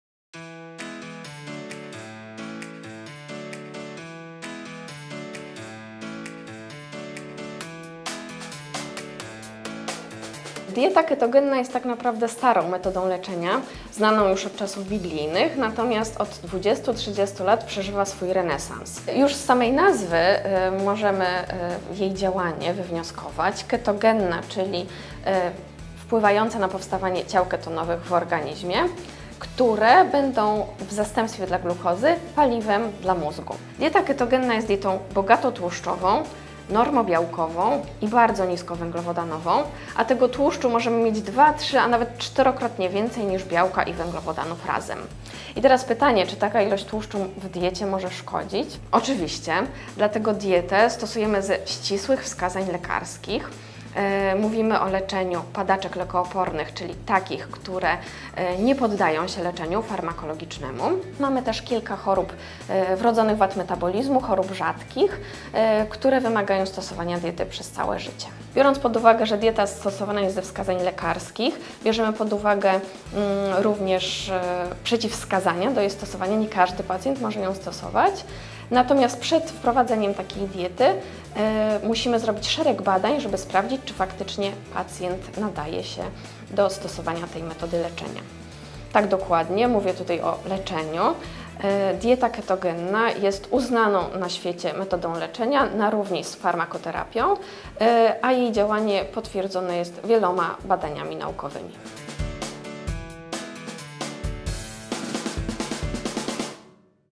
Dieta ketogenna - faktyczne zastosowanie – Ewa Ehmke vel Emczyńska-Seliga
Przy jakich wskazaniach zdrowotnych dieta ketogenna spełnia swoją rolę? Czym się charakteryzuje? Wysłuchaj opinii naszego eksperta